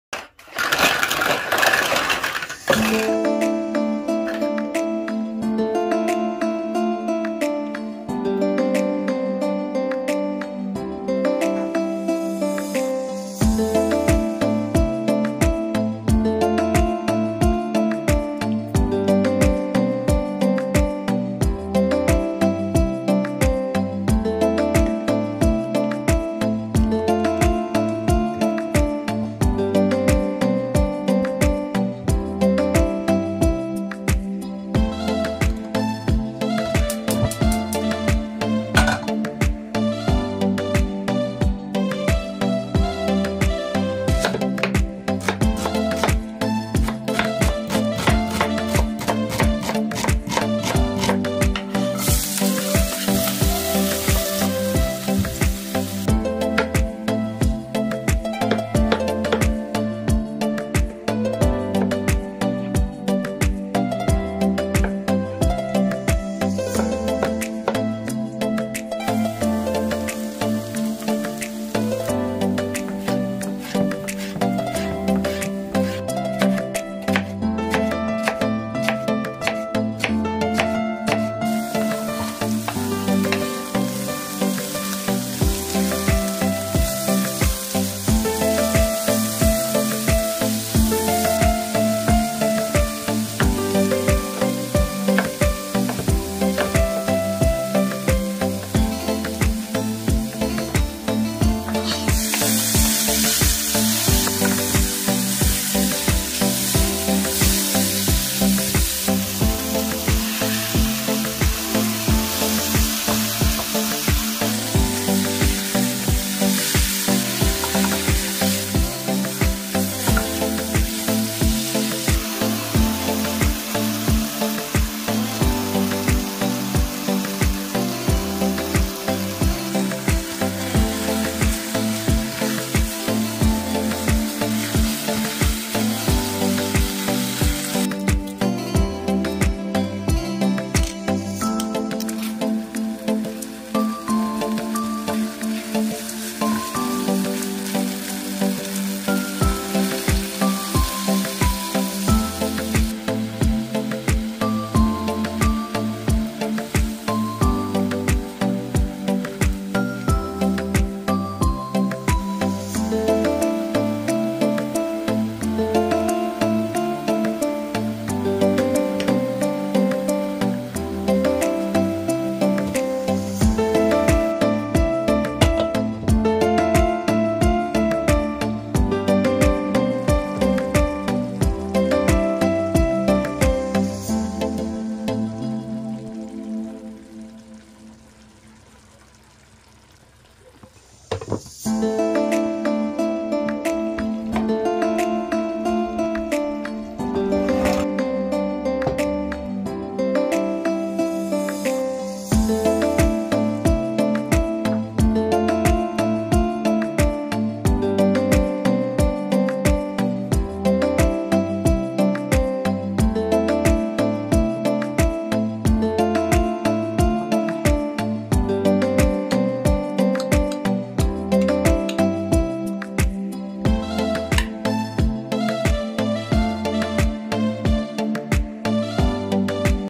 Каннеллони с Сочной начинкой Простое в приготовлении блюдо на Сковороде
Каннеллони

Помидор черри

Моцарелла

Растительное масло

Грибы 5 шт

Сладкий перец 1 шт

Томатная паста 1 с.л

Соль 2 с.л

Чёрный молотый перец 1/2 ч.л

Сыр